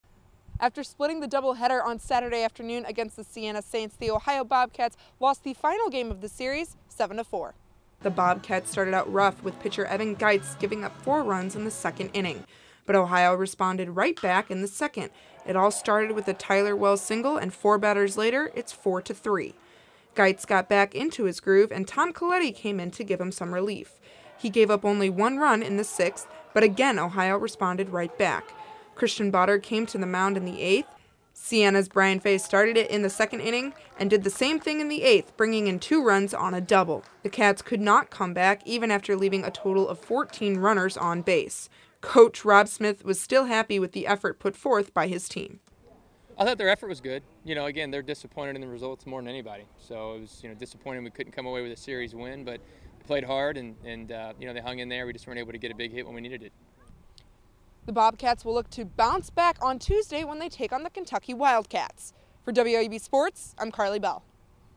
Ohio Baseball: Bobcats Drop Series Against Siena
The Ohio Bobcats split the double header on Saturday but lost the series finale on Sunday, 7-4. Karli Bell has more.